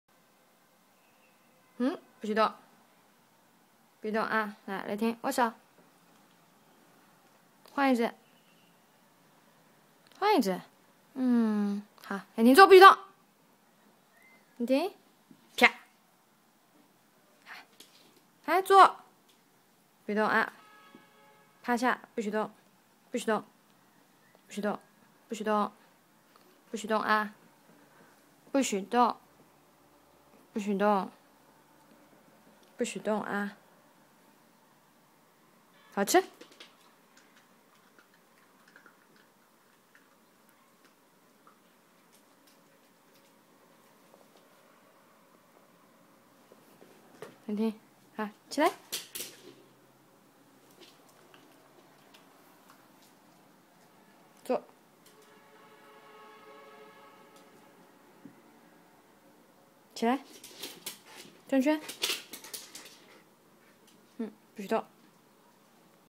Lightening playing tricks~~~ handshaking, pretending dead, turning circles~~~
Lightening is a two-year-old MINIATURE SCHNAUZER who is friendly and sweet as well as handsome!